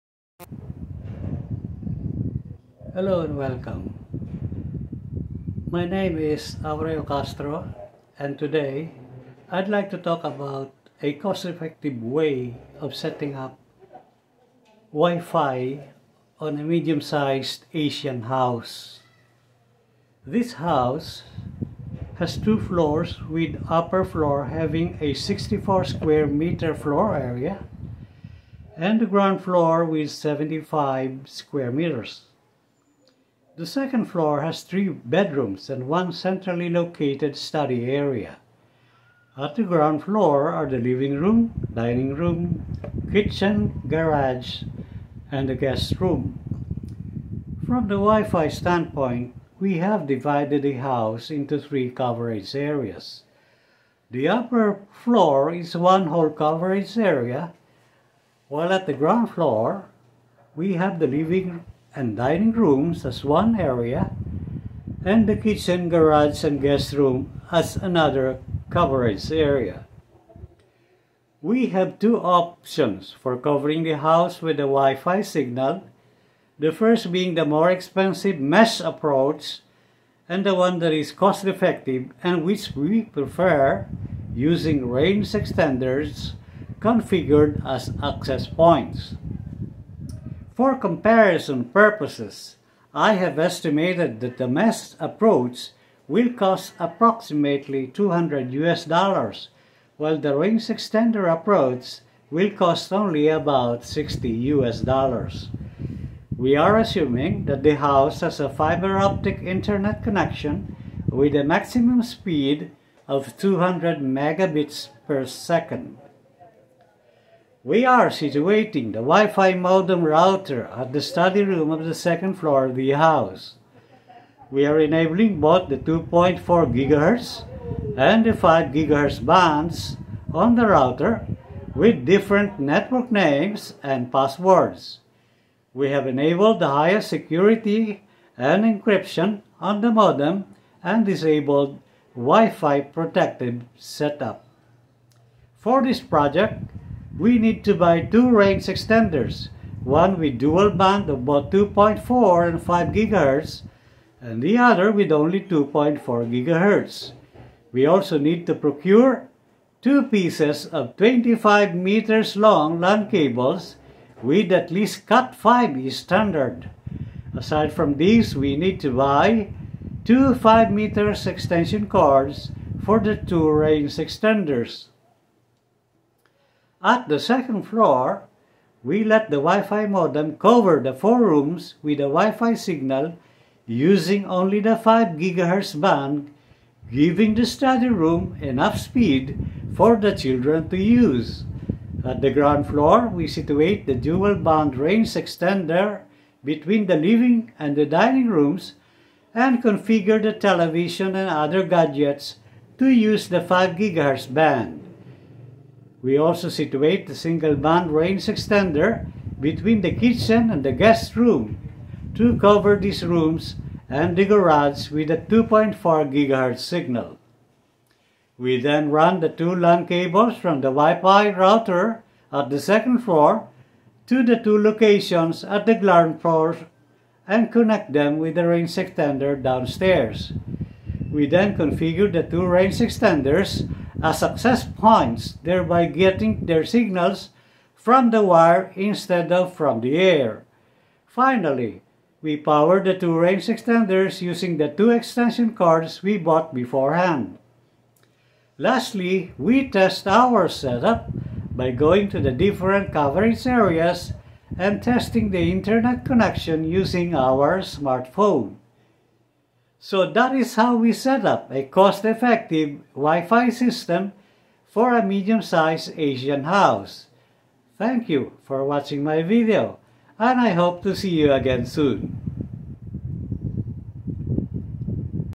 Asian House Wifi Setup
A Typical Wifi Setup for a Medium Sized Asian House. Cost effective way using range extenders configured as access points. Cheaper than the mesh wifi system. Use 5 Ghz for the study room and living room. Cover kitchen, garage, and guest rooms with 2.4 Ghz signal. Different network names and passwords. Use LAN cables to connect range extenders to the modem router. Cheap and inexpensive way of setting up wifi for a typical house in Asia.

Check out this TP-Link Deco S7 Home Mesh Wifi System from Lazada

Check out my article Looking for a Place to Live at Google Sites

If any of the thoughts or ideas on this page had been helpful to you, please donate any amount to the author Thanks.